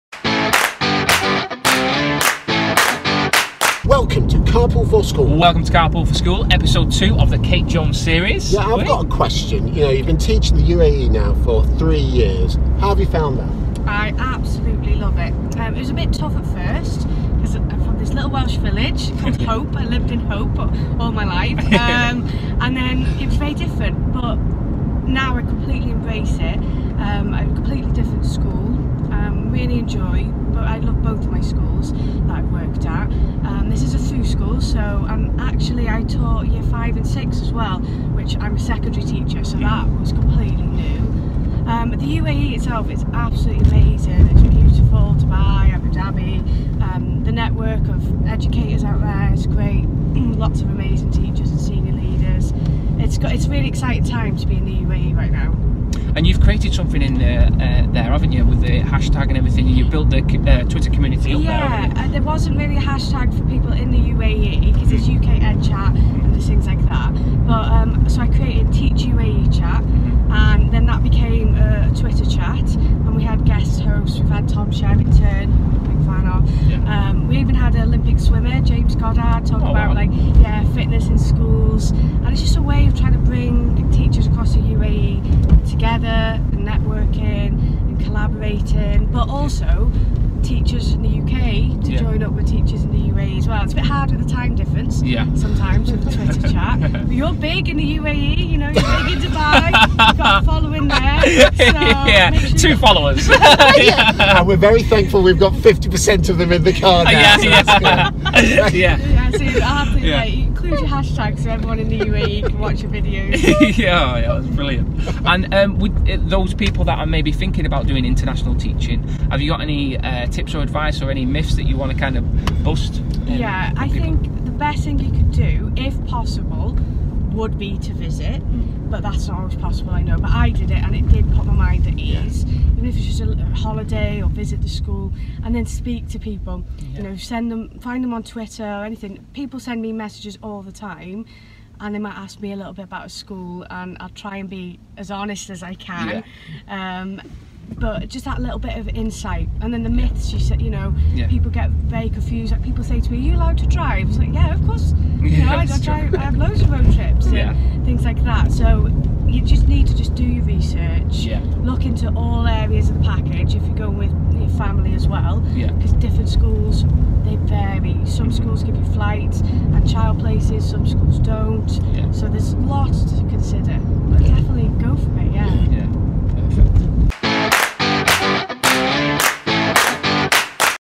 Episode Two of the Kate Jones series - Teaching internationally
Kate talks about her experience teaching internationally and gives some advice regarding it.